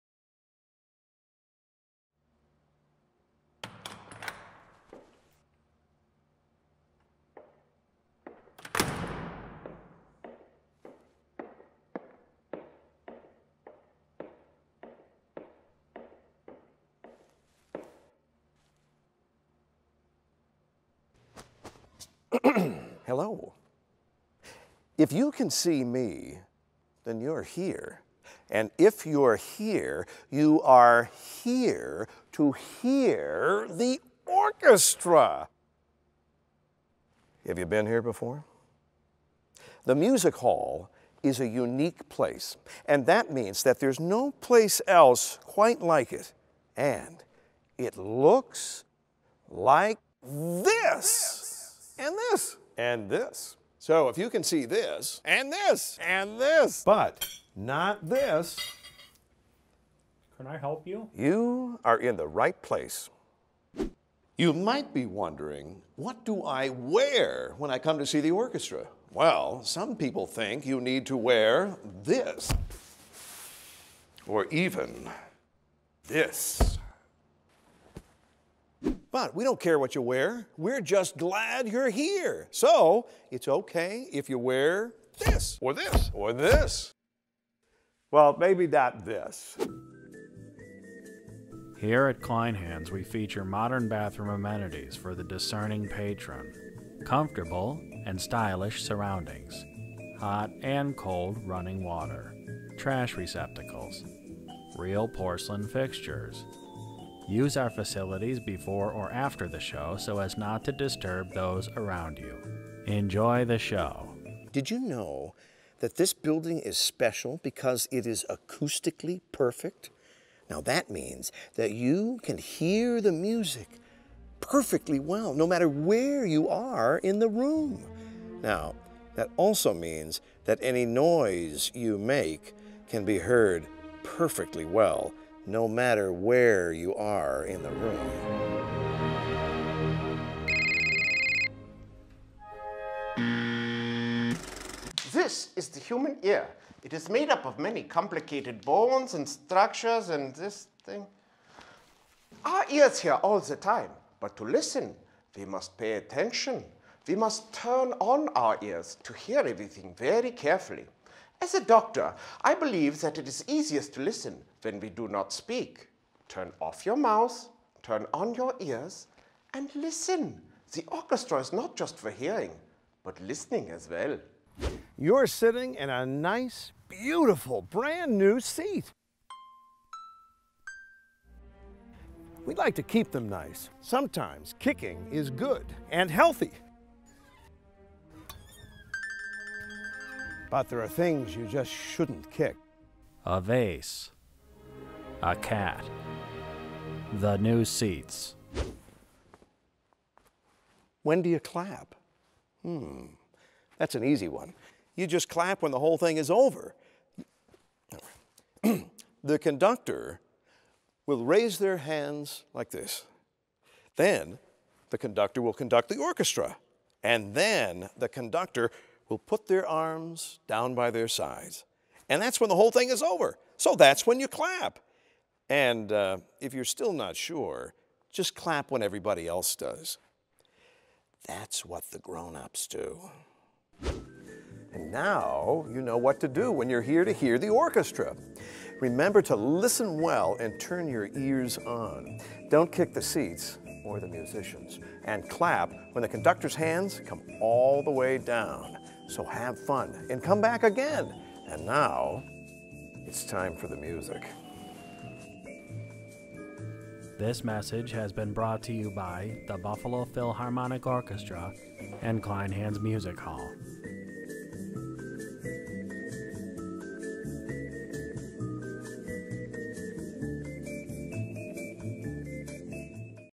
BPO Young People's Concert Prep
This video will be shown prior to the Buffalo Philharmonic's youth concerts and BPOKids concerts to prepare our students and families for the concert experience.  Enjoy!

Created for the BPO by Cumbo, Inc.